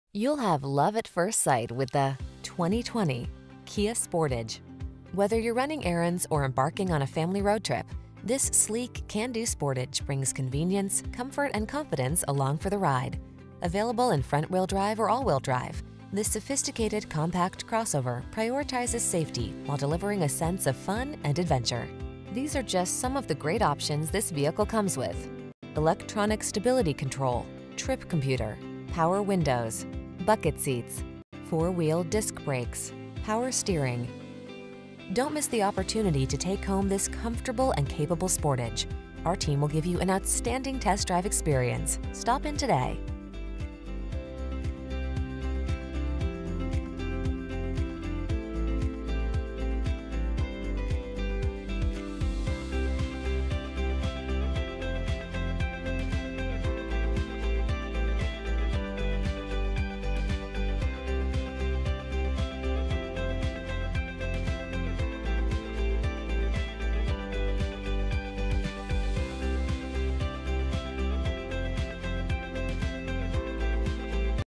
2020 Kia Sportage Greer, Spartanburg, Greenville, Gaffney, Easley, SC 61587A
Steel Gray Used 2020 Kia Sportage available in Greer, South Carolina at Kia of Greer. Servicing the Spartanburg, Greenville, Gaffney, Easley, SC area.

14345 E. Wade Hampton Blvd